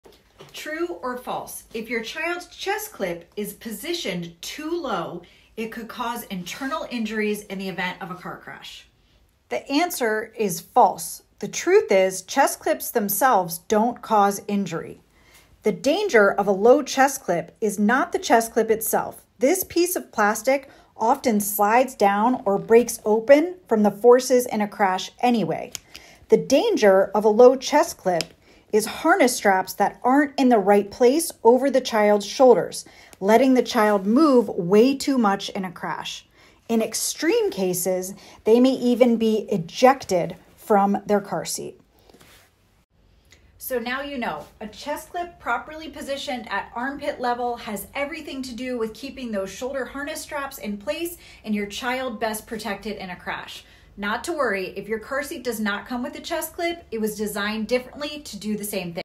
One of the biggest myths!
The danger of a low chest clip is not the chest clip itself - this piece of plastic often slides down or breaks open from the forces in a crash anyway. The danger of a low chest clip is harness straps that aren’t in the right place over the child’s shoulders, letting the child move way too much in a crash. In extreme cases, they may even be ejected from their seat. So yes, we need that chest clip to be right at armpit level, but not for the reason you might have thought!

Note: chest clips aren’t commonly used outside the US and Canada. Around the world, car seats use other design and engineering methods to ensure proper harness strap positioning. But if your car seat comes with a chest clip, it’s important to use it correctly!